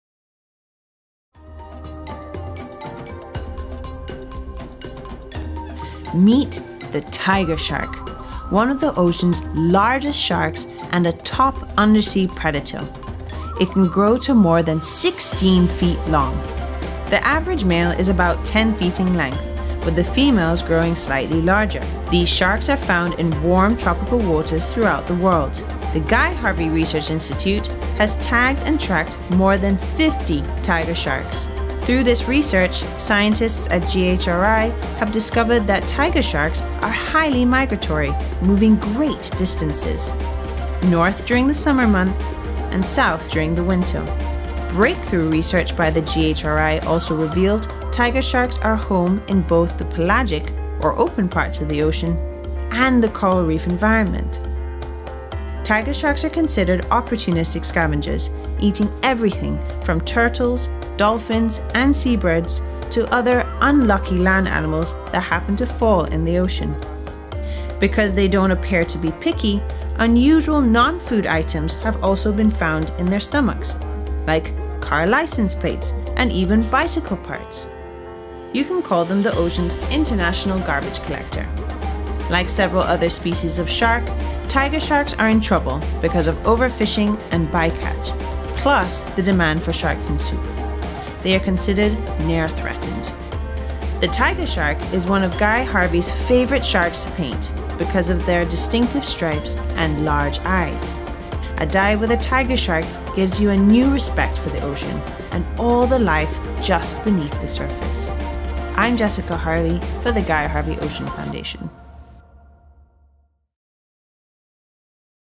Expedition Notebook: The Tiger Shark
Meet Guy Harvey's favorite shark to paint, the Tiger Shark. Jessica Harvey looks at breakthrough information on this formidable undersea predator.